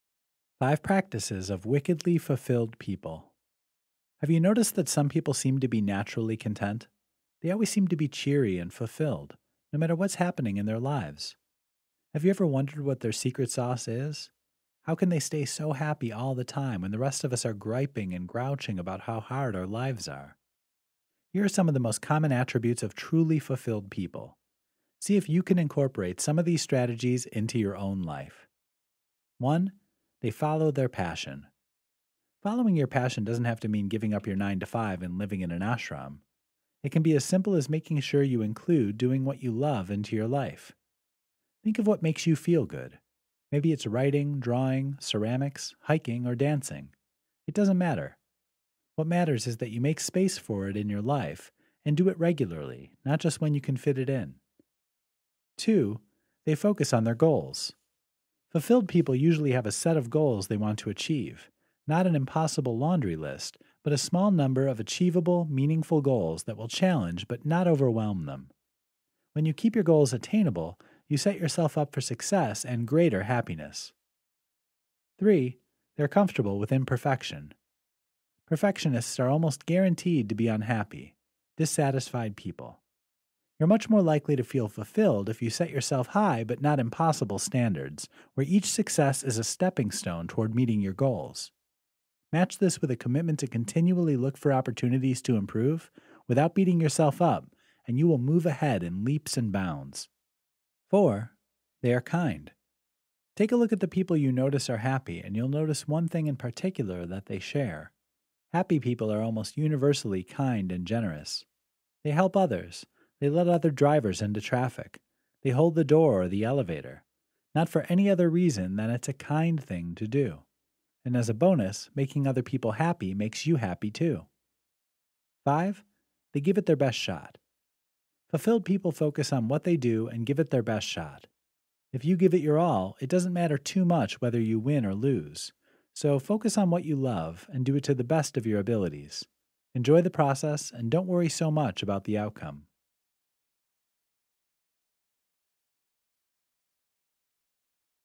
5 Practices of Wickedly Fulfilled People
5 Practices of Wickedly Fulfilled People

Have you noticed that some people just seem to be naturally content? They always seem to be cheery and fulfilled, no matter what’s happening in their lives. Have you wondered what their secret sauce is? How can they stay so happy all the time when the rest of us are griping and grouching about how hard our lives are?

Here are some of the most common attributes of truly fulfilled people. See if you can incorporate some of these strategies into your own life.

1. They follow their passion

Following your passion doesn’t have to mean giving up your 9 to 5 and living in an ashram. It can be as simple as making sure you include doing what you love into your life. Think of what really makes you feel good – maybe it’s writing, drawing, ceramics, hiking or dancing, it doesn’t matter. What matters is that you make space for it in your life. And do it regularly, not just when you can fit it in.

2. They focus on their goals

Fulfilled people usually have a set of goals they want to achieve. Not an impossible laundry list, but a small number of achievable, meaningful goals that will challenge but not overwhelm them. Keeping your goals attainable sets you up for success and greater happiness.

3. They are comfortable with imperfection

Perfectionists are almost guaranteed to be unhappy, dissatisfied people. You’re much more likely to feel fulfilled if you set yourself high but not impossible standards, where each success is a stepping stone towards meeting your goals. Match this with a commitment to continually look for opportunities to improve, without beating yourself up, and you will move ahead in leaps and bounds.

4. They are kind

Take a look at the people you notice are happy, and you’ll notice one thing in particular that they share. Happy people are almost universally kind and generous. They help others, they let other drivers into the traffic, they hold the door or the elevator. Not for any other reason than it’s a kind thing to do. And as an added bonus, making other people happy makes you happy too.

5. They give it their best shot

Fulfilled people focus on what they do and give it their best shot. If you give it your all, it doesn’t matter too much whether you win or lose. So, focus on what you love, and do it to the best of your abilities. Enjoy the process and don’t worry so much about the outcome.